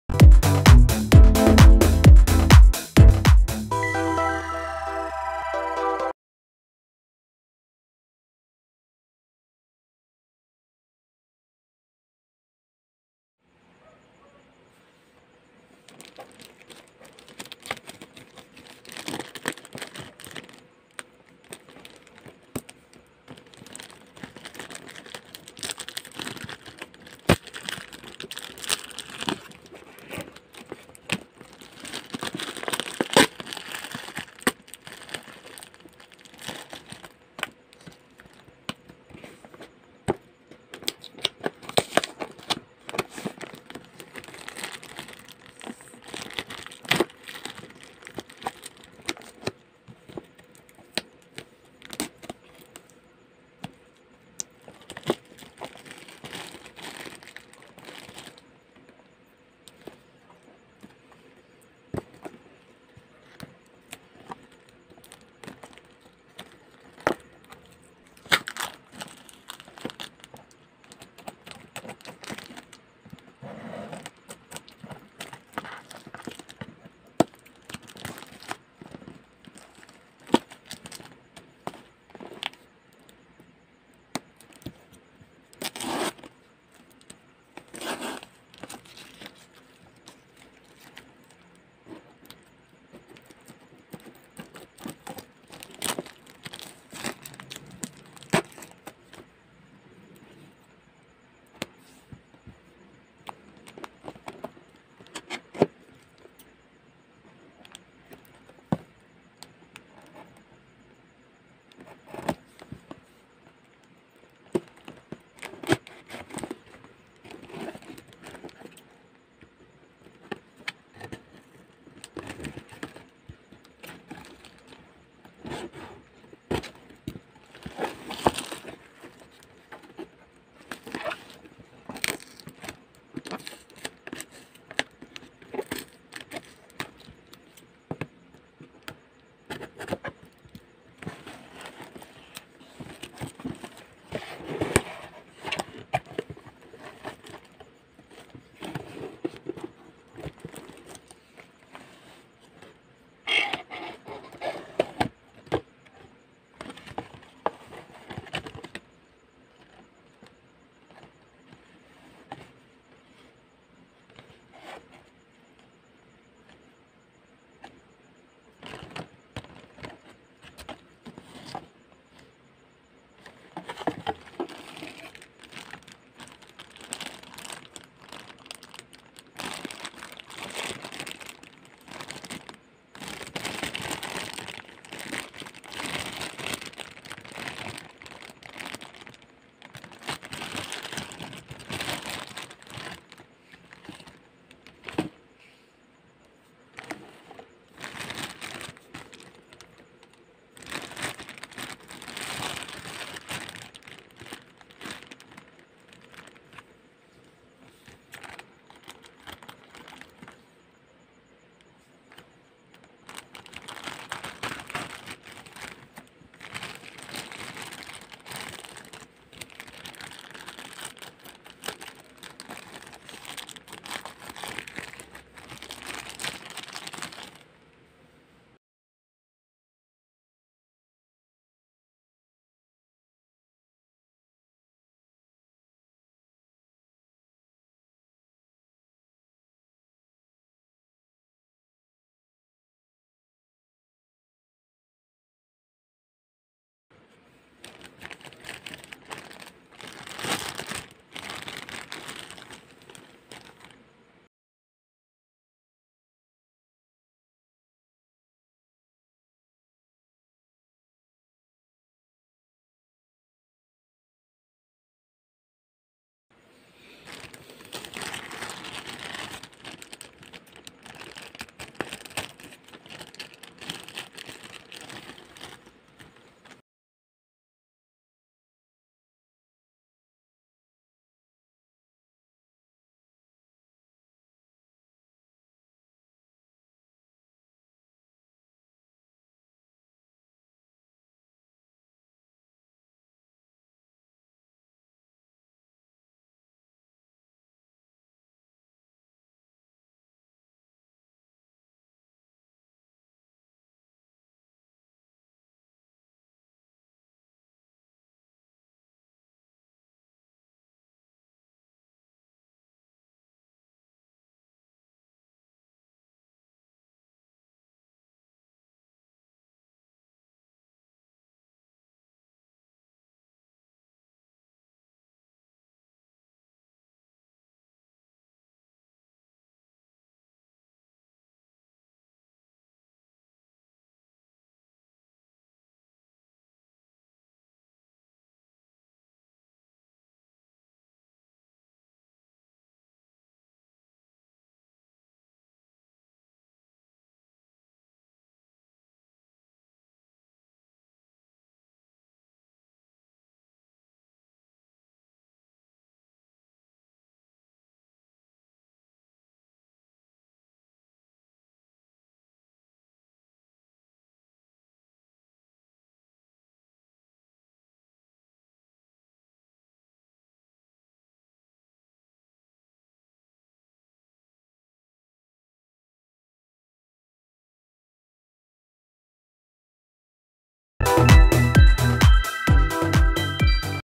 One-Punch Man DXF Genos Premium Figure | Bandai
Nahilig ako ngayon sa panunuod sa facebook Live ng mga Japan Online Live Seller ng mga anime figures. so napa mine mine mine na naman ako :)

For today unbox natin ang One-Punch Man DXF Genos Premium Figure ng Bandai.

Nabili ko itong figure sa halagang 800 pesos, maliban pa yan sa local shipping fee kapag dumating na ang item sa Pilipinas.
Mapapansin nyo na nakahiwalay yung box at yung item sa LBC box kasi karamihan sa nagse-sell ng anime figure from Japan para tipid sa shipping papuntang Pilipinas eh "tupi box - tapon clamshell" sila. Pero legit at MISB ito nung nasa live selling hahaha :D

. andami ko ng sinabi, let's watch the video na !!! :D :D :D